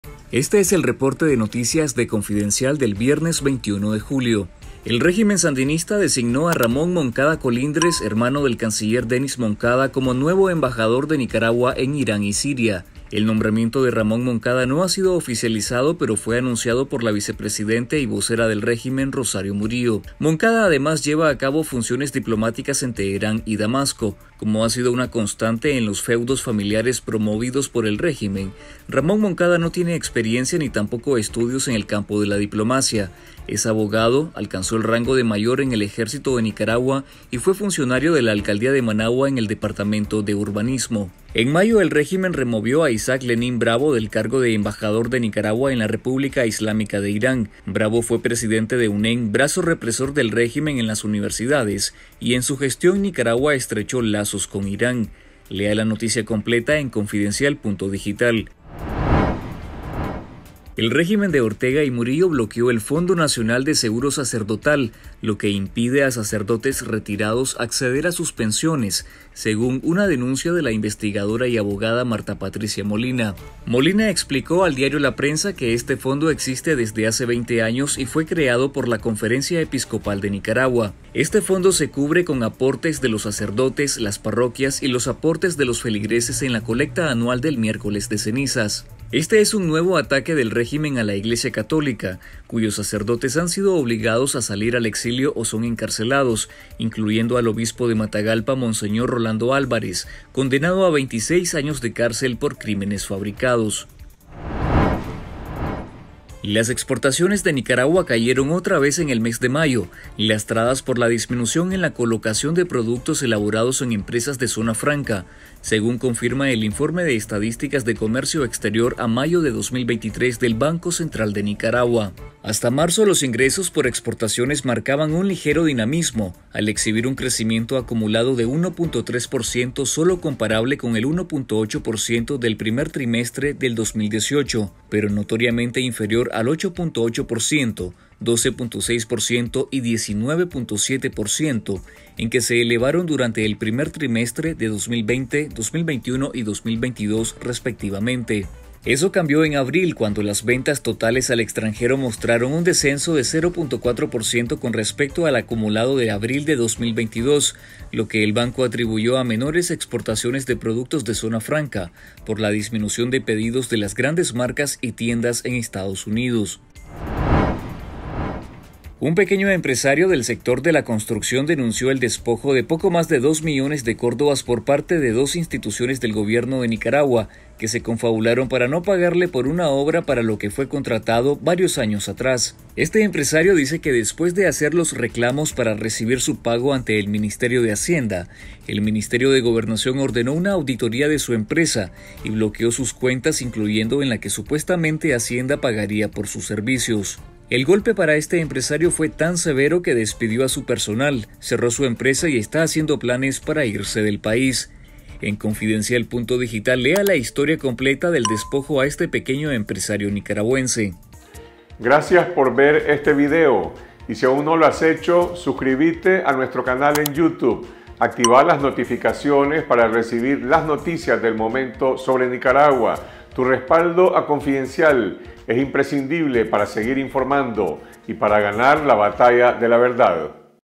Hermano del canciller Denis Moncada es nombrado embajador en Irán y Siria
El régimen sandinista designó a Ramón Moncada Colindres, hermano del canciller, Denis Moncada Colindres, como nuevo embajador de Nicaragua en Irán y Siria.
El nombramiento de Ramón Moncada no ha sido oficializado, pero fue anunciado por la vicepresidente y vocera del régimen, Rosario Murillo. Moncada, además, lleva a cabo funciones diplomáticas en Teherán y Damasco.
Como ha sido una constante en los feudos familiares promovidos por el régimen, Ramón Moncada no tiene experiencia ni tampoco estudios en el campo de la diplomacia. Es abogado, alcanzó el rango de mayor en el Ejército de Nicaragua, y fue funcionario de la Alcaldía de Managua en el departamento de urbanismo.
En mayo, el régimen removió a Isaac Lenin Bravo Jaen del cargo de embajador de Nicaragua en la República Islámica de Irán. Bravo fue presidente de la UNEN, brazo represor del régimen en las universidades, y en su gestión Nicaragua estrechó lazos con Irán.

Estas son otras noticias destacadas de la jornada en Nicaragua:

- Régimen congela fondo de retiro de los sacerdotes nicaragüenses.
- Banco Central confirma que las exportaciones de Nicaragua siguen cayendo.
- Empresario de la construcción relata cómo quebró por culpa del Gobierno.